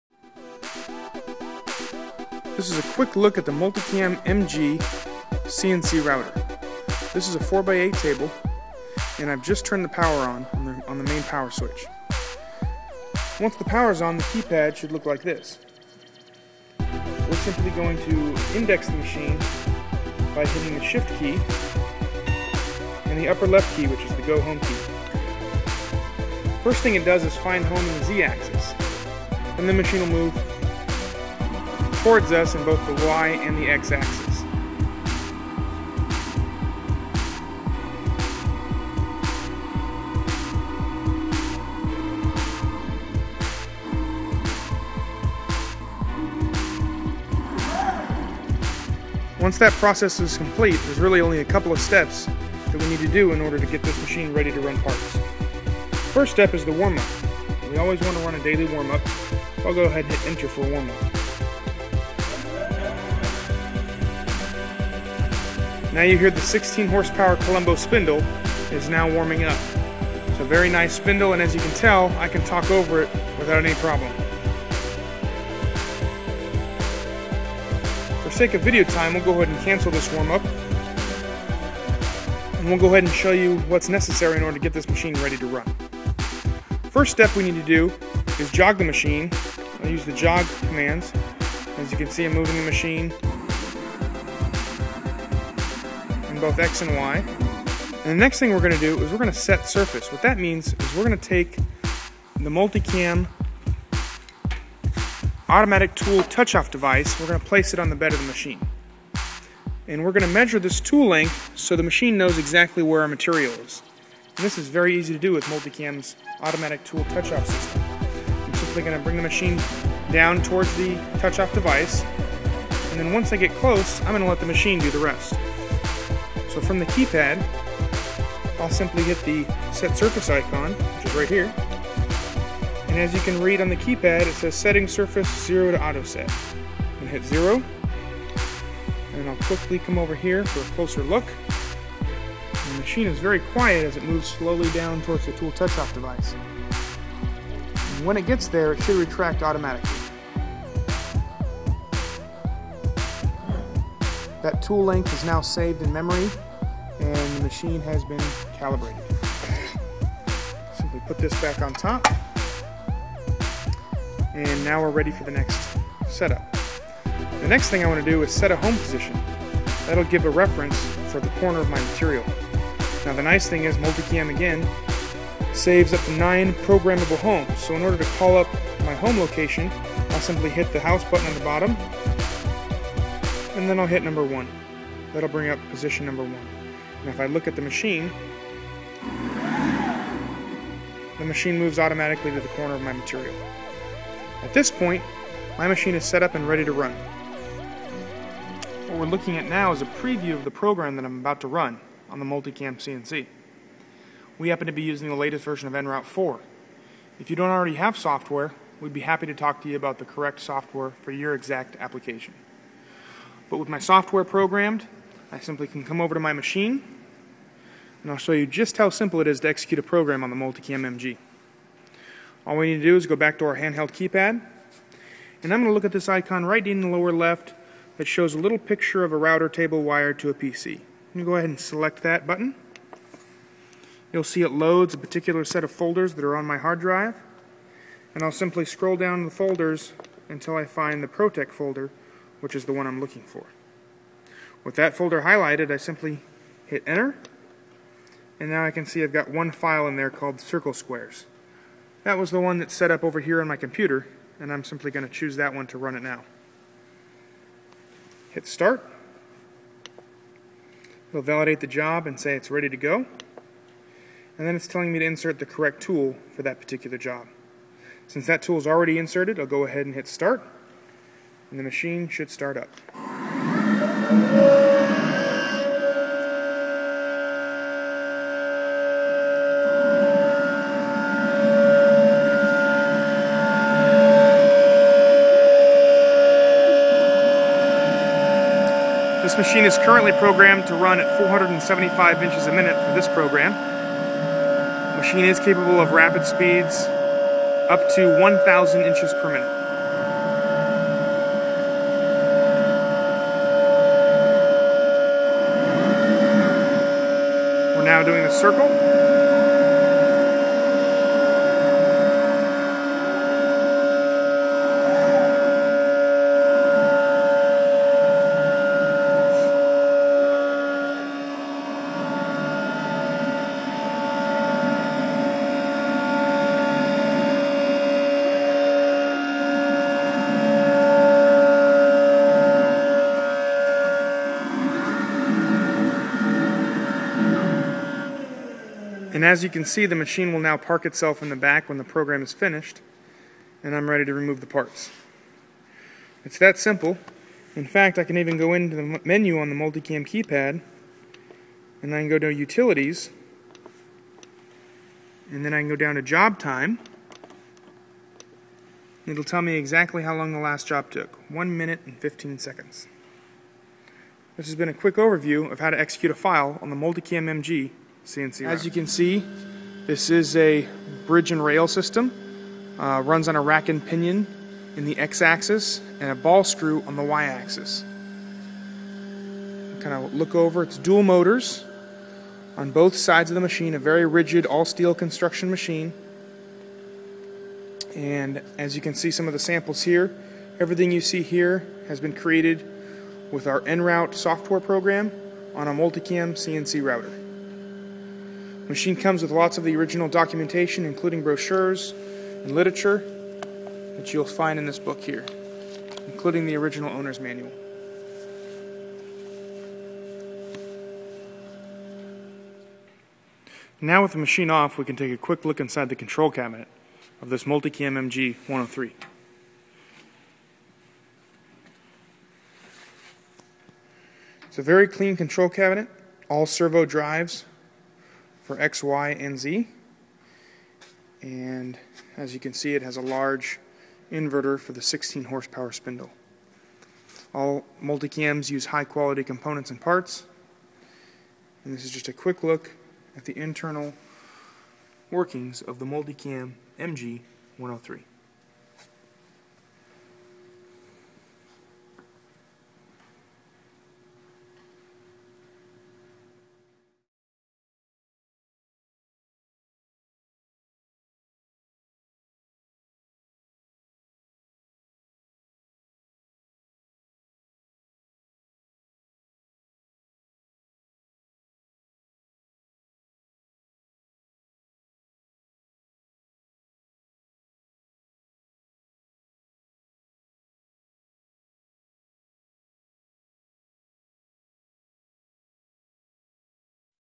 MultiCam MG 4x8 CNC Router
This machine is a late 2002 MultiCam MG CNC Router.
It has had light use and was not being utilized enough to keep.
It is available to ship before the end of the year!
Please email for more informaiton.